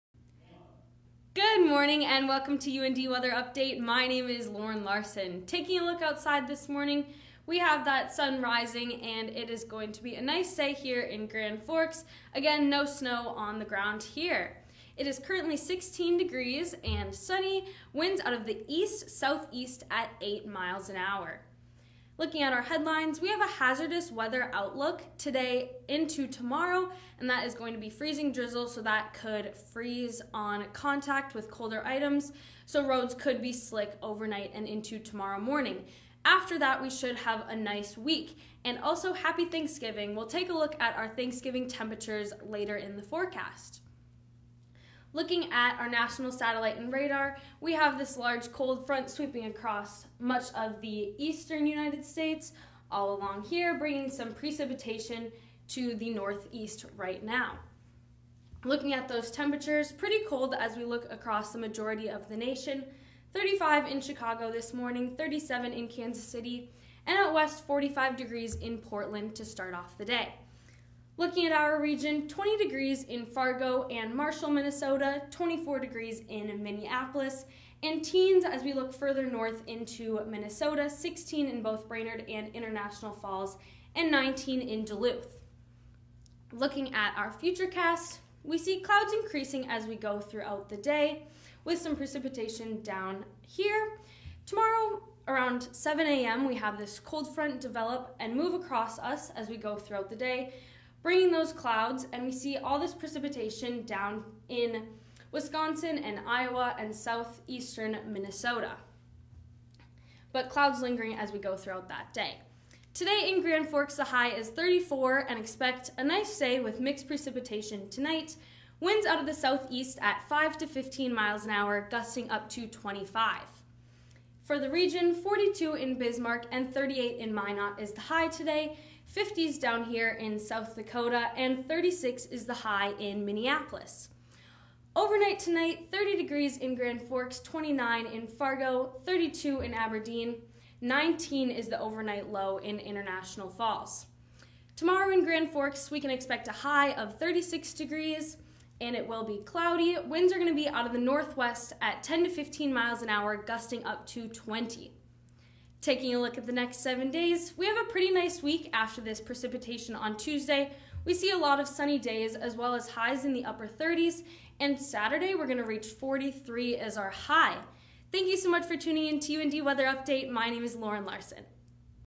Monday Regional Weather Forecast From Grand Forks
GRAND FORKS, ND (iNewZ.TV) Your Monday regional weather forecast from UND"s Atmospheric Sciences.